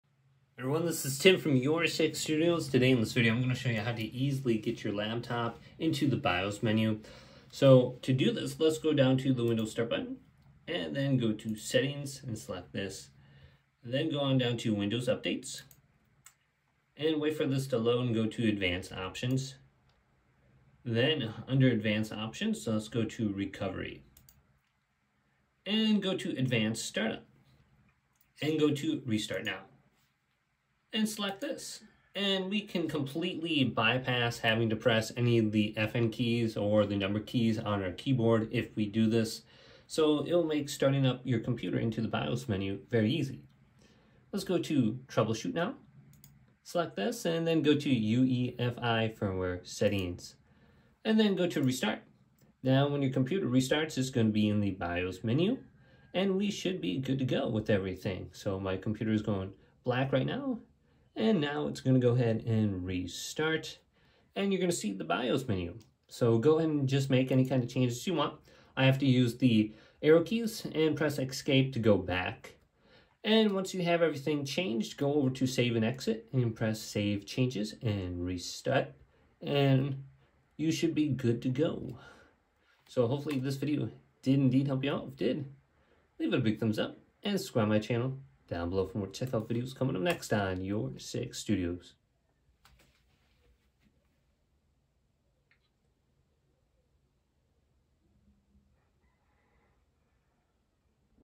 How to Start your Laptop in the BIOS menu on Windows 10 or 11 PC
To start your laptop in the BIOS menu on a Windows 10 or 11 PC, follow these steps:

Method 1: Using a Keyboard Shortcut (During Boot)
Shut Down Your Laptop:

Turn off your laptop completely.
Power On and Press BIOS Key:

Turn on your laptop and immediately start pressing the BIOS key repeatedly. Common keys include F2, Del, F10, or Esc, depending on your laptop's manufacturer.
Keep pressing until the BIOS menu appears.

Method 2: Through Windows Settings (Windows 10 & 11)
Open Settings:

Click on the Start menu, then select Settings.
Navigate to Recovery Options:

Go to System - Recovery (Windows 11) or Update & Security - Recovery (Windows 10).
Advanced Startup:

Under the Advanced startup section, click Restart now.
Choose Boot Options:

Your PC will restart and show a blue screen with options. Select Troubleshoot - Advanced options - UEFI Firmware Settings, then click Restart.
BIOS Menu:

Your laptop will restart and take you directly into the BIOS menu.